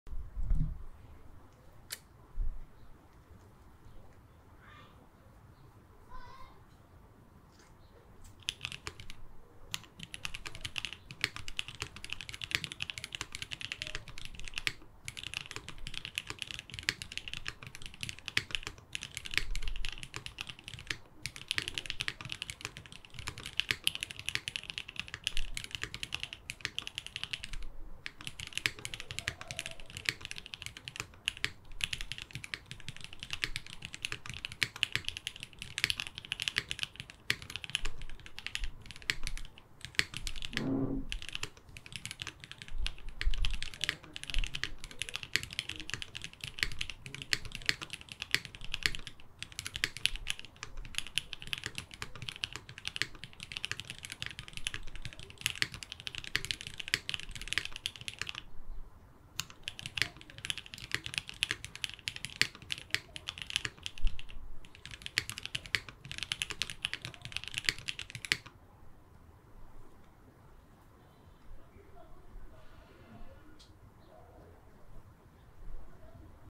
Catto Bakeneko - Katze60 w/ Gateron X Soundtest
The drunken Catto is here
- Keyboard: Katze60
- Plate: Carbon fibre
- Switches: Gateron X
- Stabilizers: Owlab
- Keycap: GMK Shoko
- Cable: CableMod
- PCB Foam: No
- Case Foam: No

The board was assembled using one of my good friends' service, highly recommended